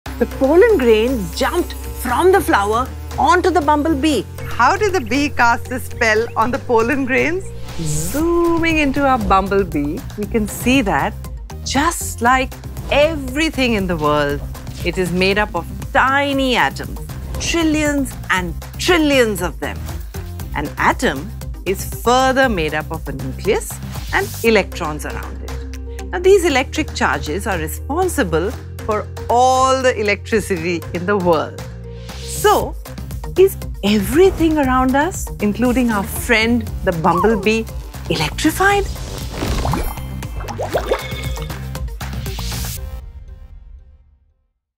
How do Bees Help in Pollination with Electricity?
How do bees help in plant reproduction? They attract pollen grains using static electricity! Watch the latest ‘Now We Know video’ NOW on Byju’s channel to find out

🔷 BYJU's APP is for FREE 🤩 go for it!!!